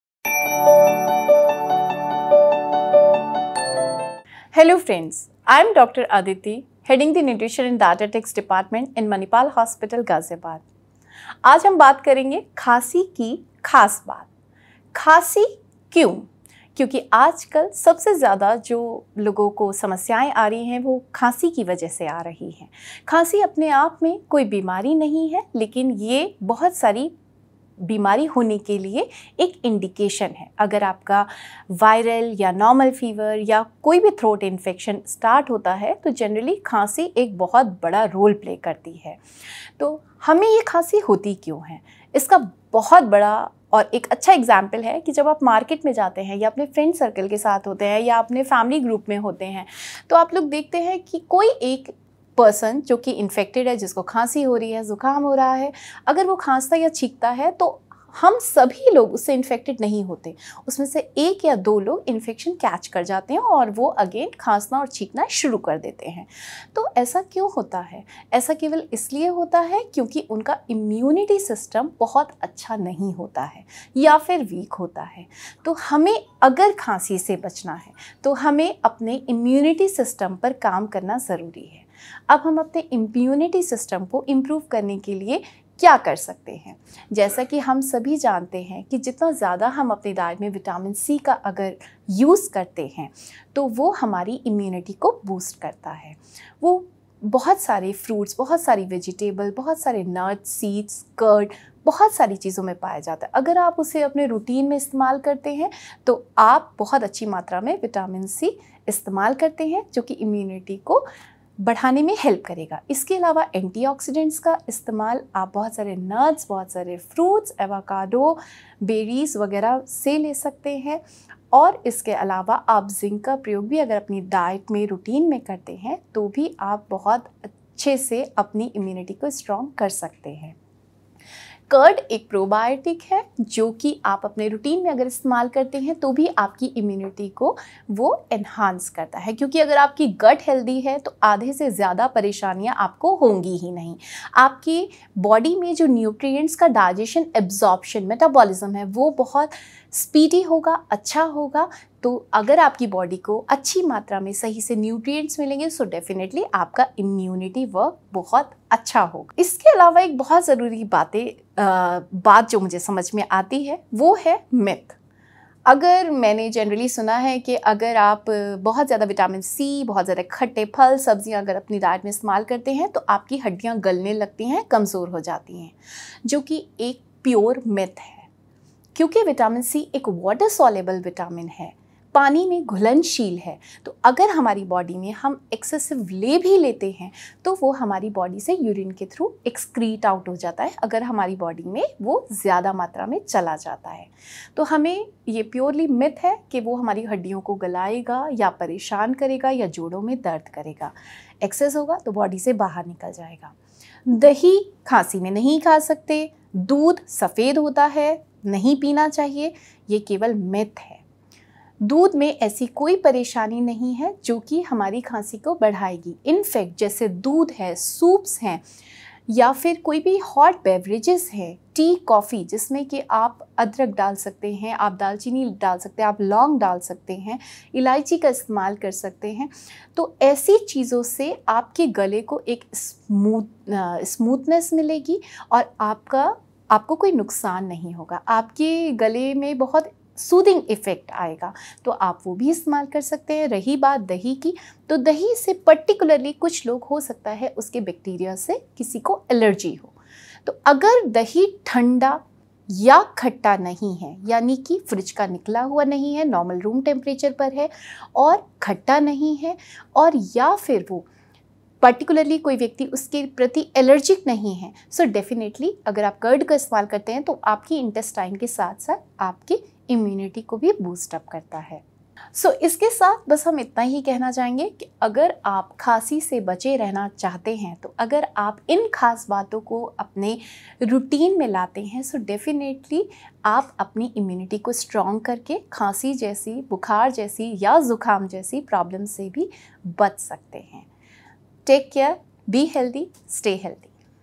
Dietary Advice | Dr. Aditi Sharma | Manipal Hospital Ghaziabad
Dr. Aditi Sharma, Consultant - Nutrition & Dietician, Manipal Hospital Ghaziabad, talks about the causes of coughing and explains the diseases it could indicate. Watch the video as Dr. Sharma debunks a few myths and shares prevention against cough.